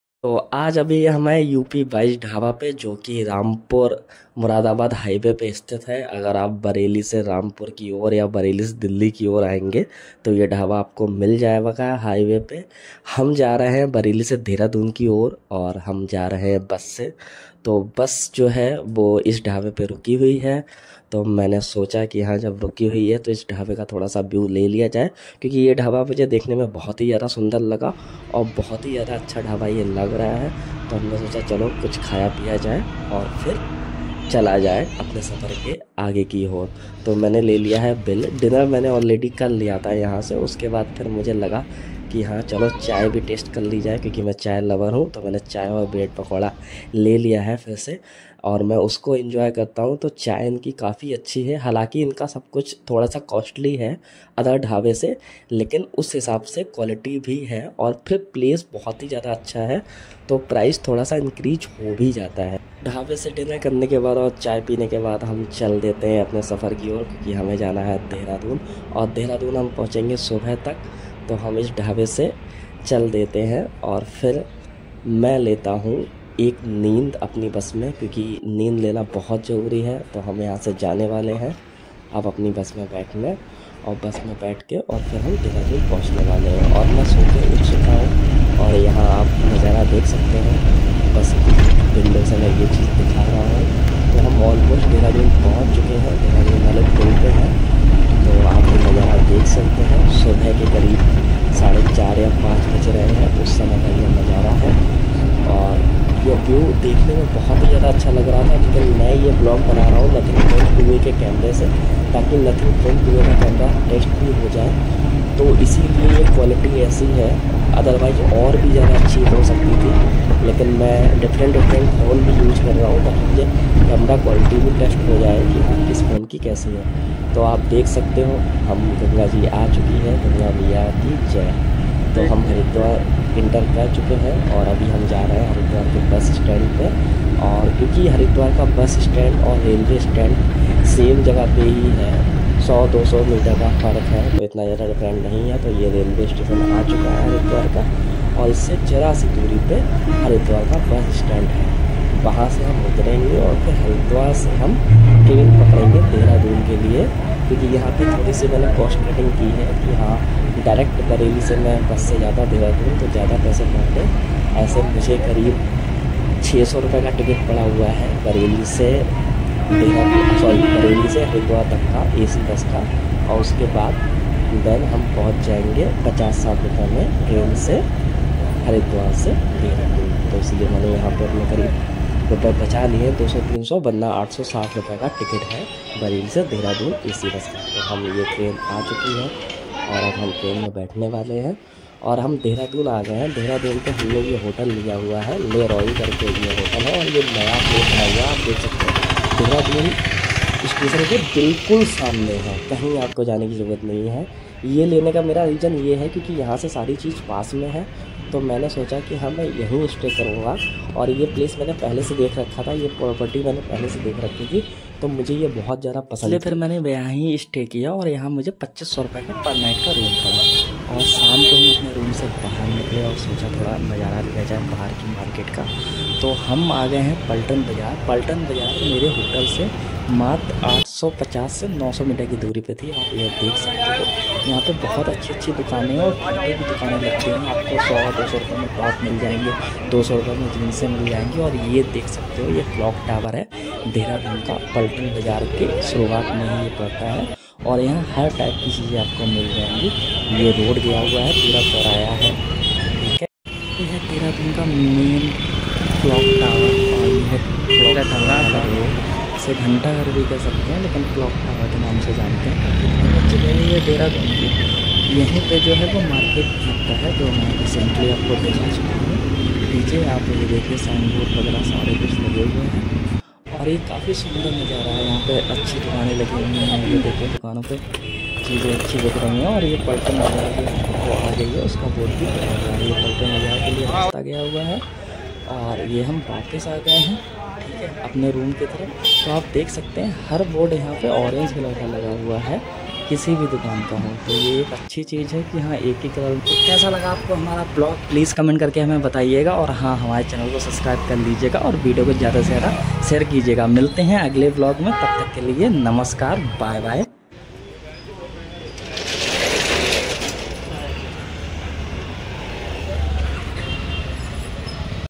Dehradun Travel Vlog With Nothing Phone 2a Camera
Hello Friends is vlog me Maine dehradun city ko dikhaya hai aur sath me Is vlog ki jo recoding ki Hain wo Nothing Phone 2a k camera se ki gyi hai ager aapko yeah video pasand aaye to please Humare channel kl subscribe karein.